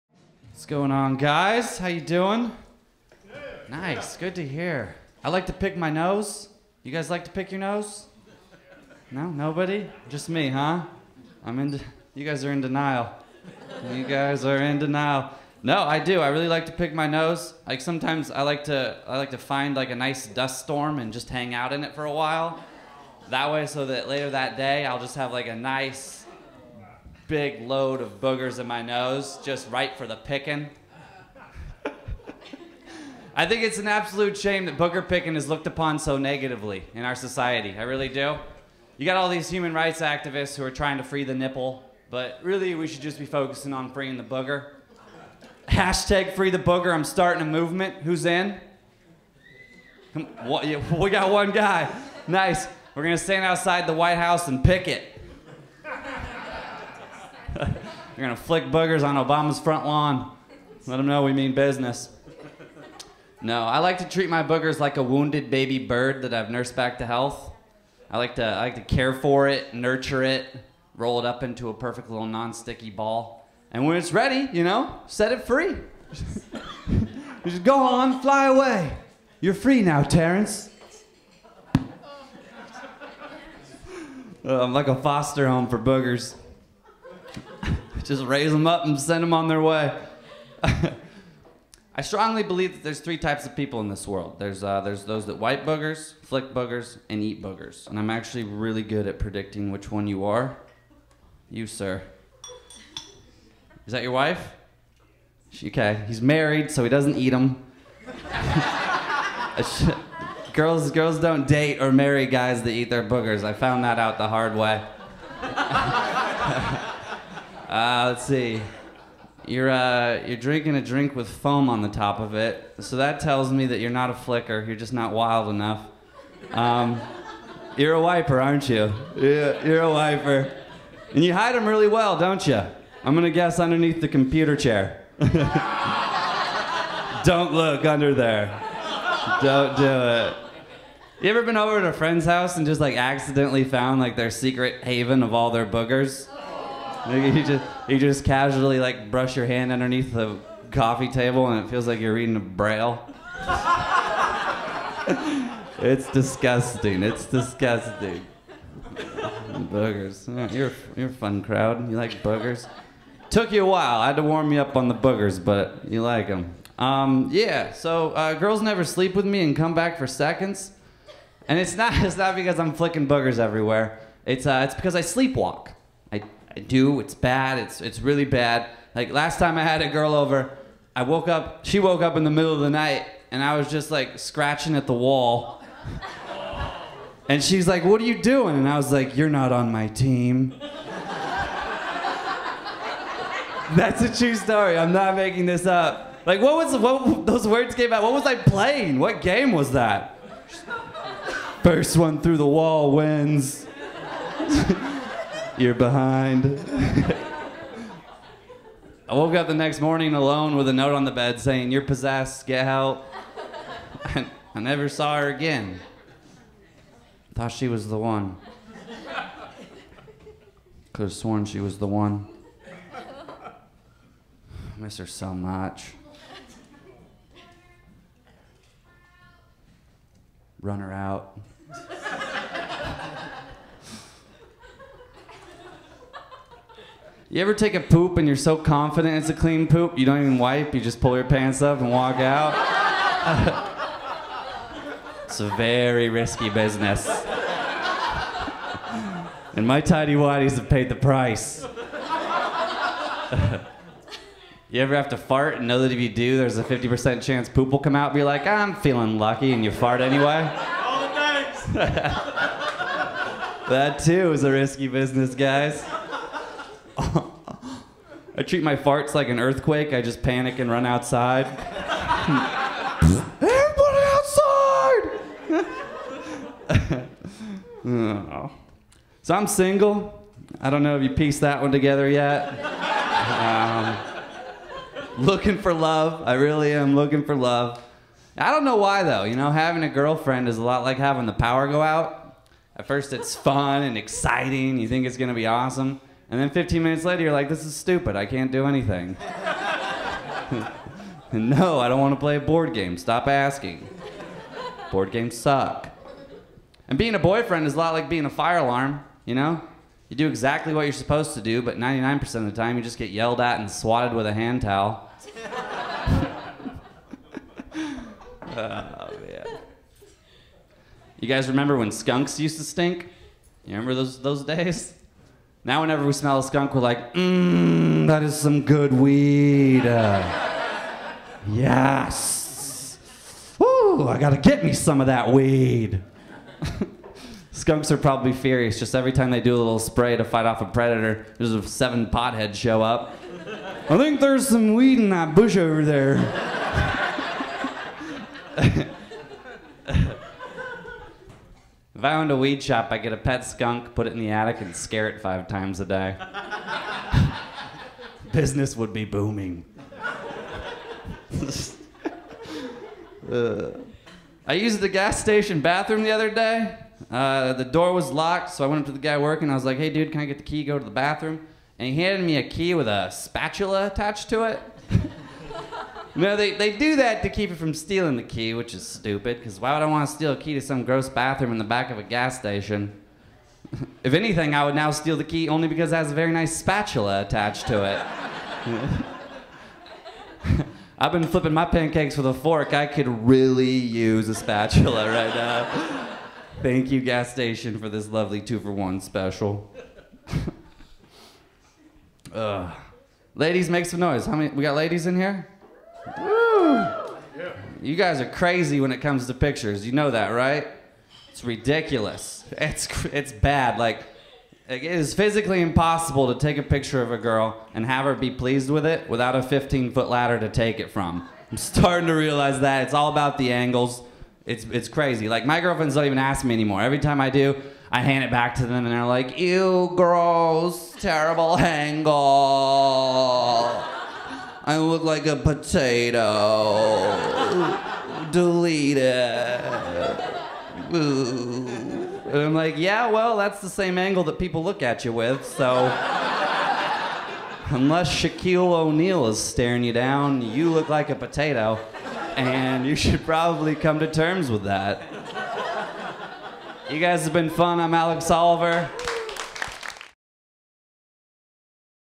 Alex Oliver: Booger Picking Full Stand Up | Comedy Time
Comedian Alex Oliver is trying to start a movement for booger picking in this full stand up set from 2016.